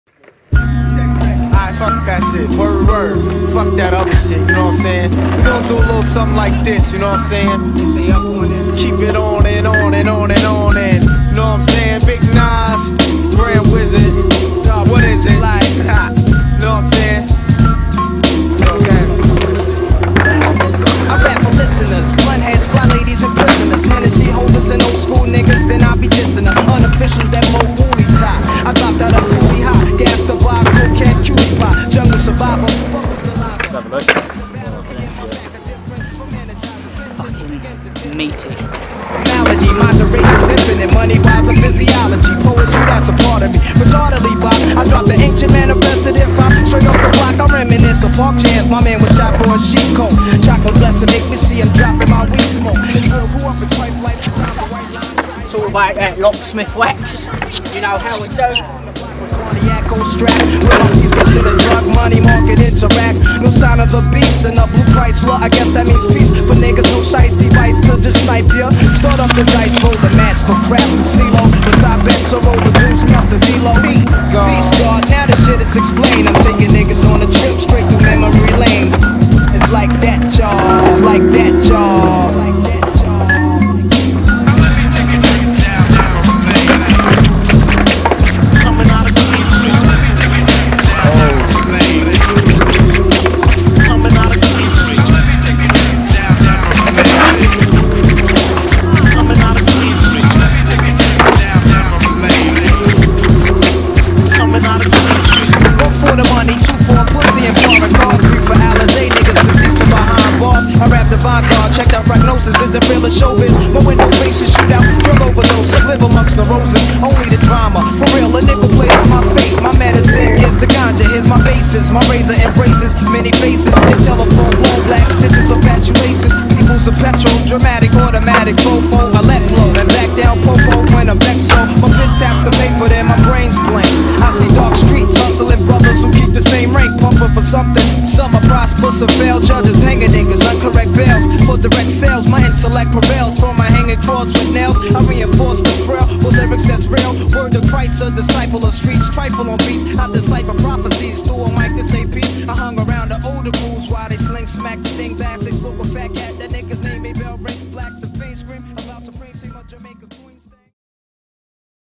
Locksmith Skateboards "Back in Kingston"
Kingston sessions featuring Jamie (Jay) Harris and Pele Castillo, a few sweet bangers in there. Be sure to check out some of our other edits and watch out for more content dropping Jan/Feb 2015!

Filmed and Edited by Anthony Mitson.

Give our Website a click: locksmithskateboards.co.uk